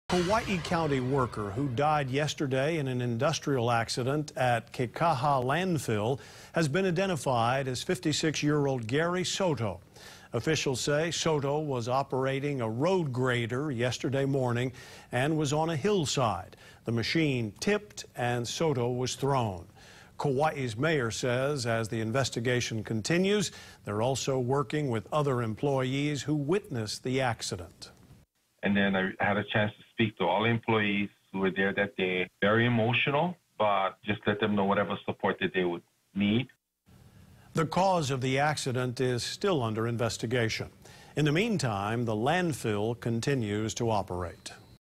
Kekaha Landfill worker killed while operating road grader
Kekaha Landfill on Kauai was closed Sunday due to an employee fatality.